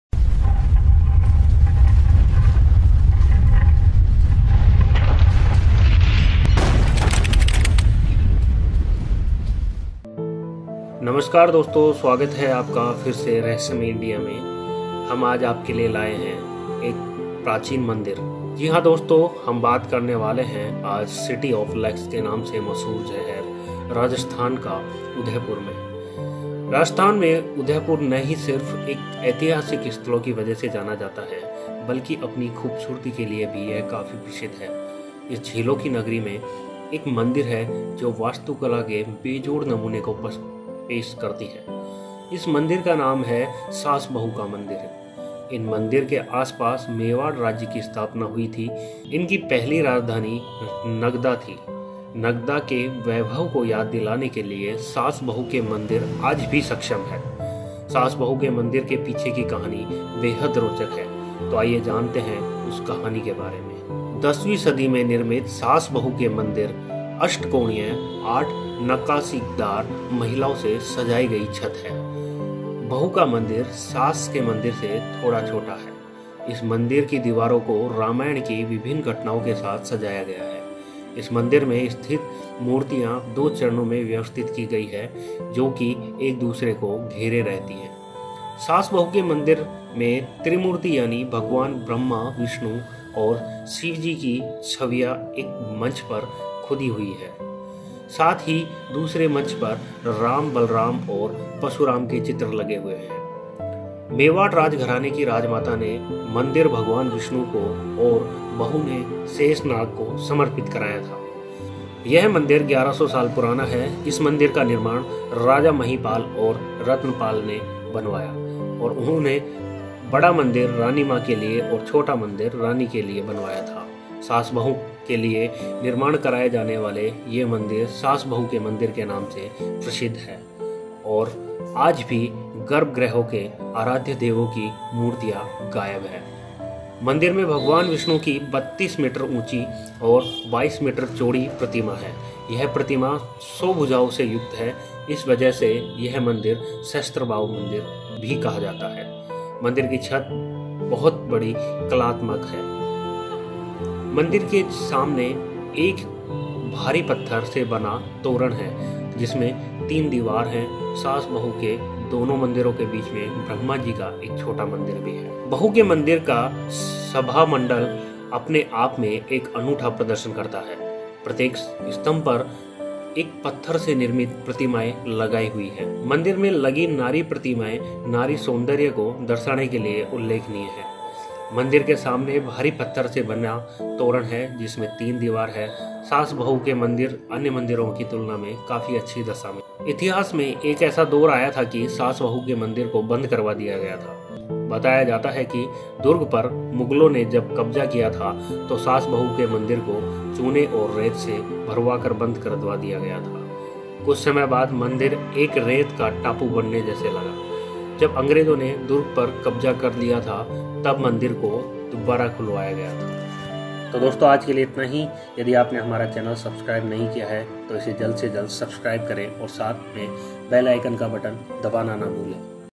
यहां है सास बहू का अद्भुत मंदिर लेकिन कहानी है बड़ी विचित्र
Welcome to Rahasya india